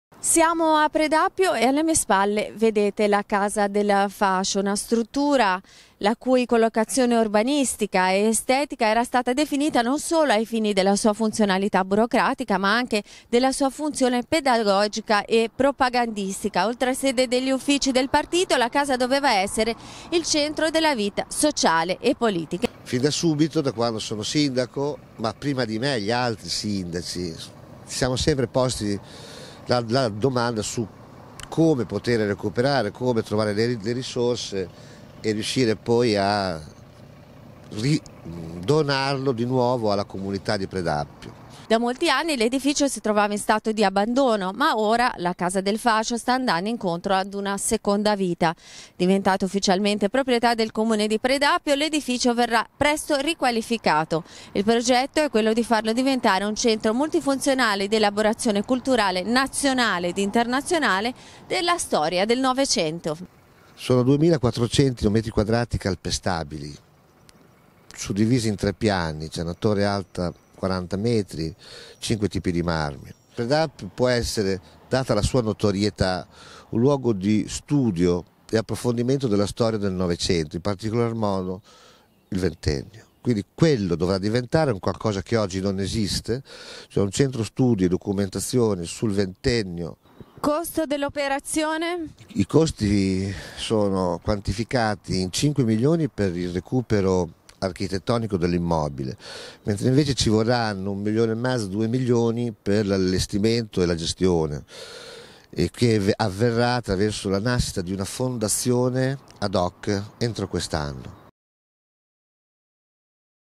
PREDAPPIO: Casa del Fascio, ora proprietà del Comune, diventerà un luogo di studi sul 900- VIDEO
La Casa del fascio di Predappio, da anni in stato di abbandono andrà incontro, nel prossimo futuro, ad una riqualificazione. Vi portiamo, con questo servizio filmato, dentro l'edificio per farvi vedere qual è il suo stato attuale e quali i progetti per il futuro.